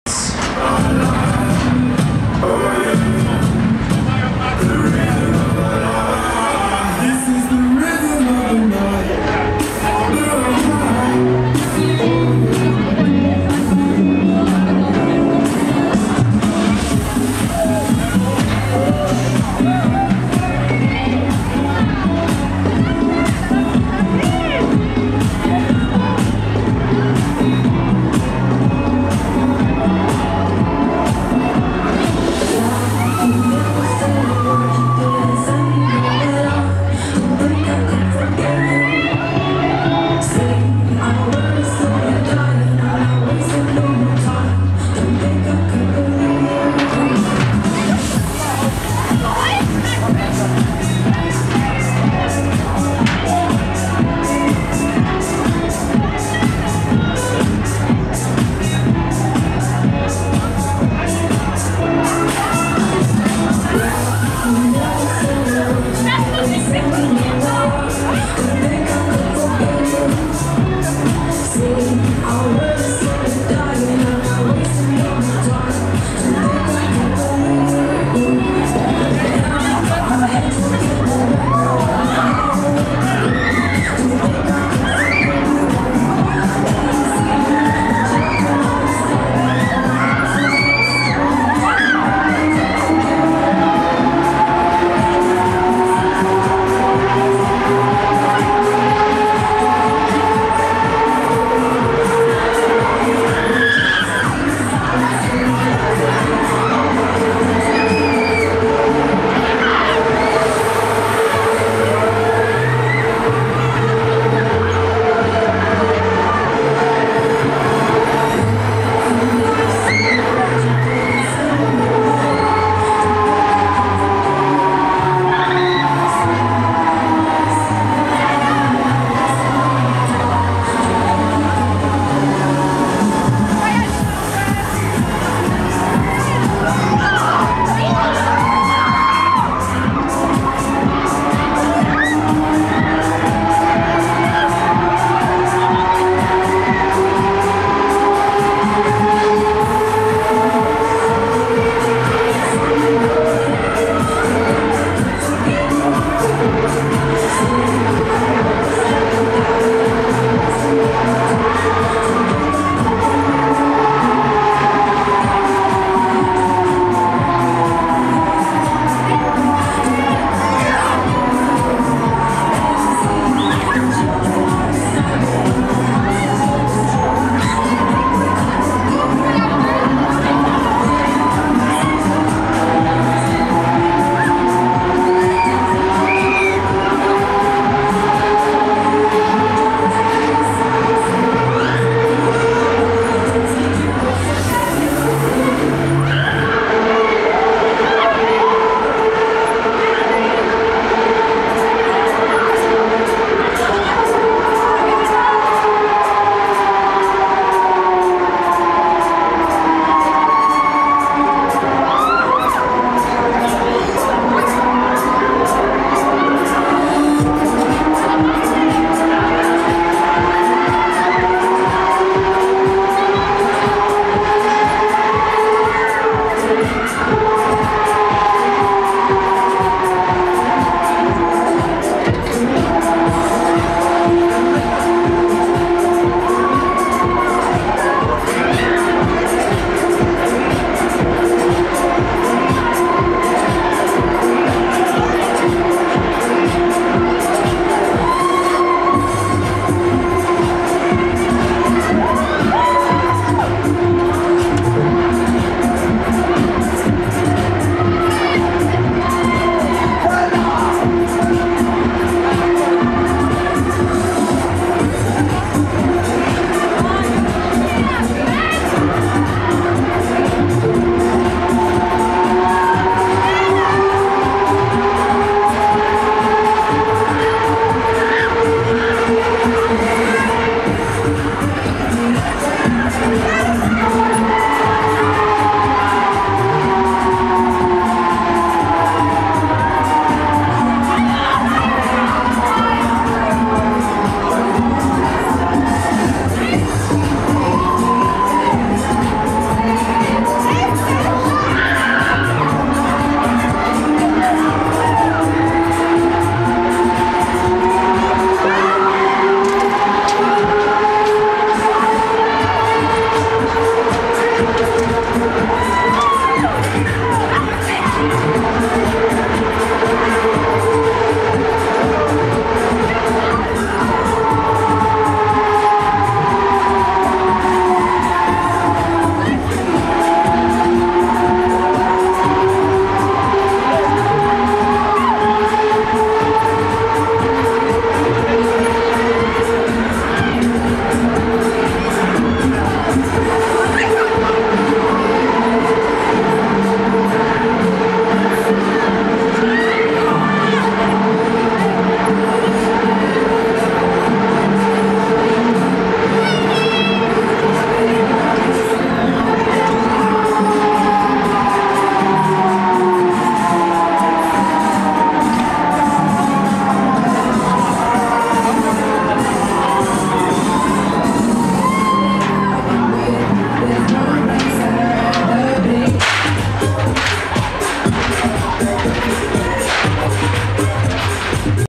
Break Dance No.2 - Bonner (Offride) Video Herbstkirmes Ahlen 2015
Break Dance No.2 von Bonner Offride Video auf der Herbstkirmes in Ahlen 2015. Break Dancer Kirmes Ahlen 2015. Fahrpreis: 3€